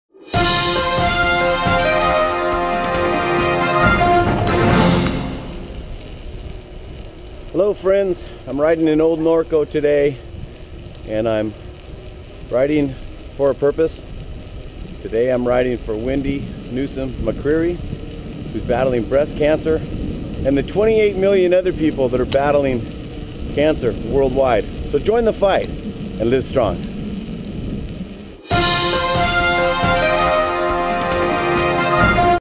Riding For A Purpose
riding this week in support of the 28 million people worldwide battling cancer. Join the fight and LIVESTRONG!